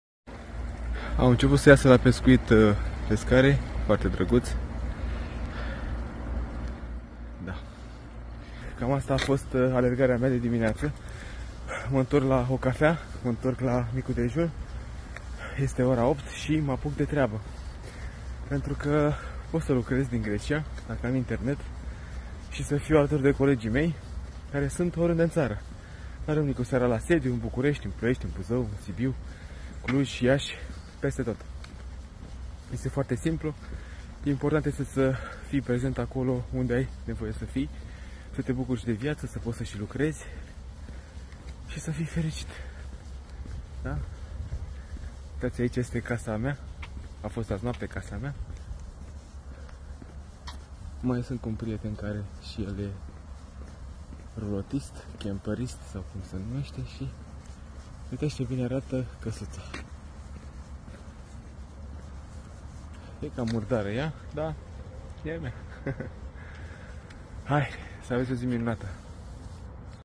Libertatea vine, când ai conexiune la internet
Ai biroul oriunde ai internet.

Ce poate fi mai frumos? Care este libertatea supremă?

Viața este așa cum ți-o faci.Cu responsabilitate și asumare.

Când am gândit Franciza Depozit Virtual am pus în ea principiile și valorile mele umane.

Constrângerea sau libertatea ar trebui sa fie o discuție cu noi insine.

Changing the game with Depozit Virtual powered by vindem-ieftin ro